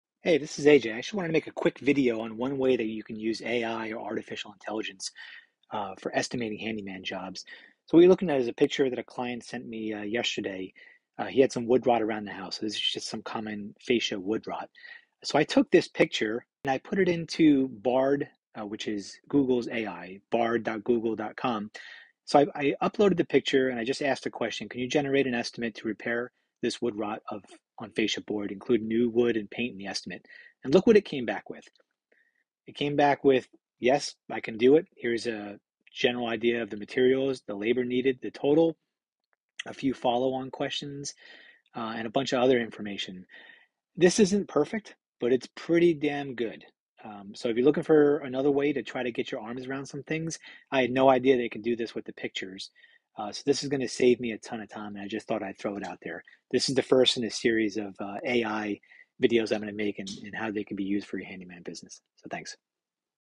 handyman|Using AI to create estimates
Bard AI can be used to generate a handyman job estimate by uploading a picture and description. Here are the steps involved:

1. Upload a picture of the job to Bard AI.
2. Provide a description of the job, including the specific tasks that need to be completed.
3. Bard AI will analyze the picture and description and generate an estimate for the job.
4. The estimate will include the cost of materials, labor, and any other expenses.

Here is an example of how Bard AI could be used to generate a handyman job estimate:

* **Picture:** A picture of a broken window.
* **Description:** The window is broken and needs to be replaced. The window is 3 feet by 3 feet and is made of wood.
* **Estimate:** Bard AI would estimate the cost of the job to be $200. This includes the cost of the window, labor, and other expenses.

Here are some additional benefits of using Bard AI to generate handyman job estimates:

* **Accuracy:** Bard AI is trained on a massive dataset of handyman jobs, so it can accurately estimate the cost of a job.
* **Speed:** Bard AI can generate an estimate in a matter of seconds, so you don't have to wait days or weeks to get a quote.
* **Convenience:** You can generate an estimate from anywhere, at any time, using your computer or mobile device.

If you are looking for a way to get accurate and quick handyman job estimates, Bard AI is a great option.